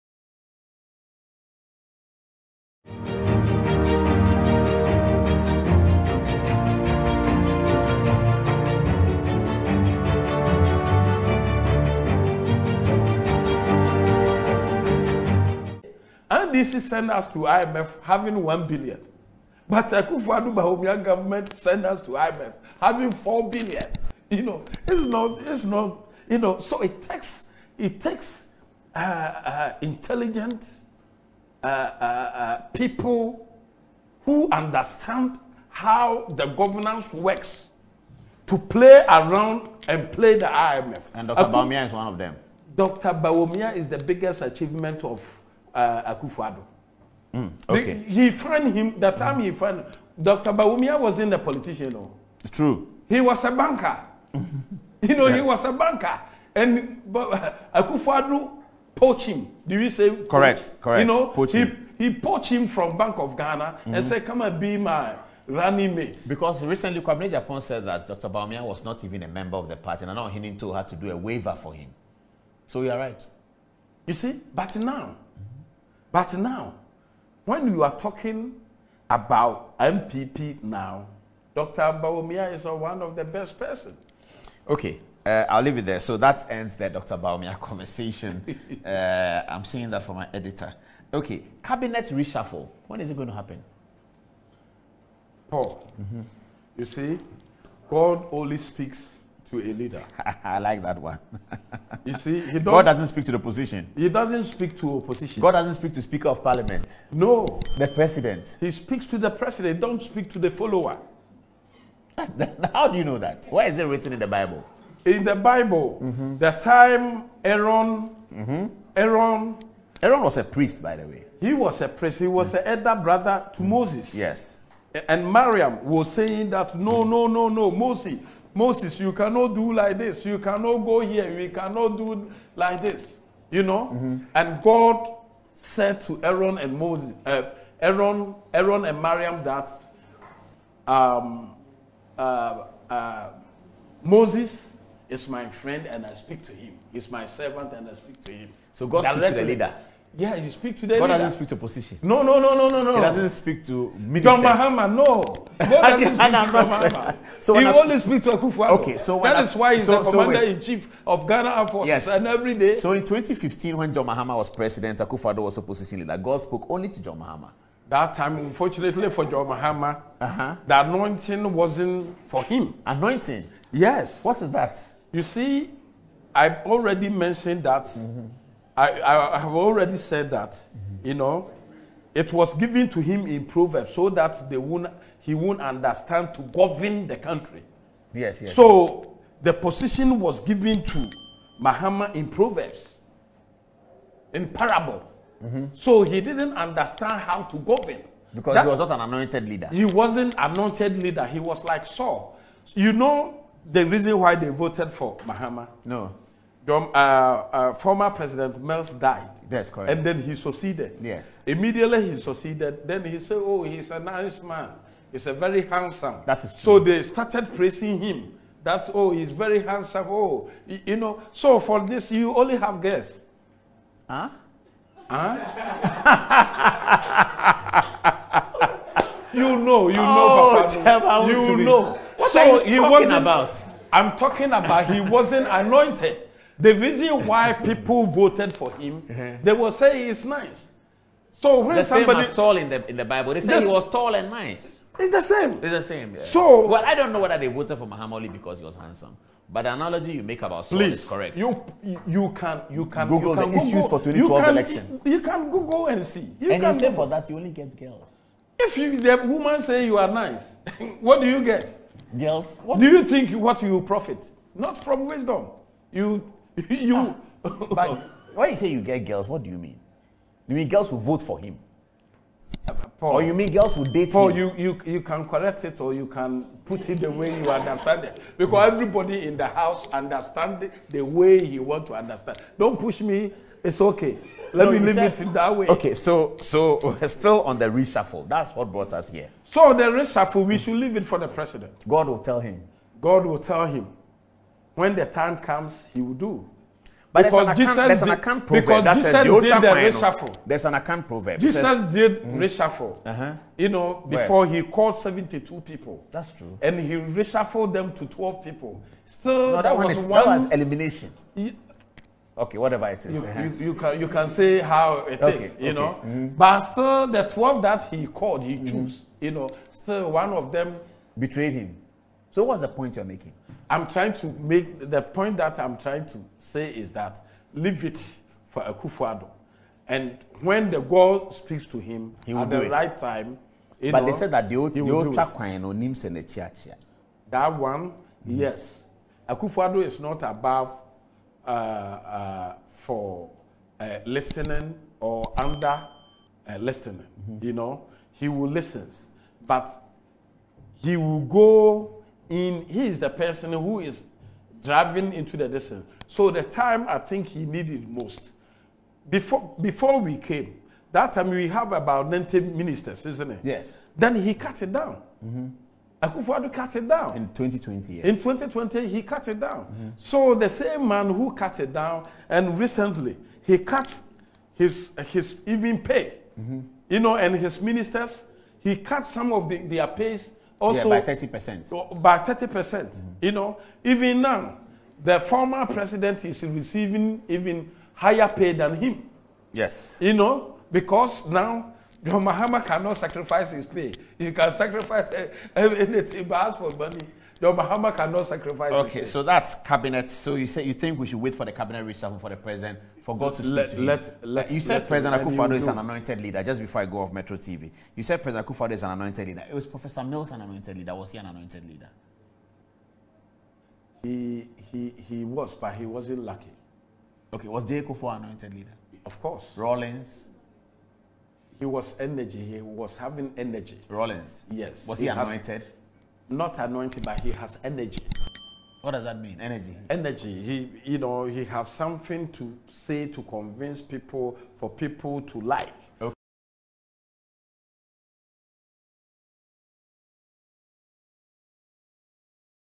THIS IS WHY AKUFFO ADDO IS BETTER THAN MAHAMA- ACCORDING WONTUMI
JOHN MAHAMA WASN'T AN ANOINTED PRESIDENT- CHAIRMAN WONTUMI EXPLAINS WHY AKUFFO ADDO IS BETTER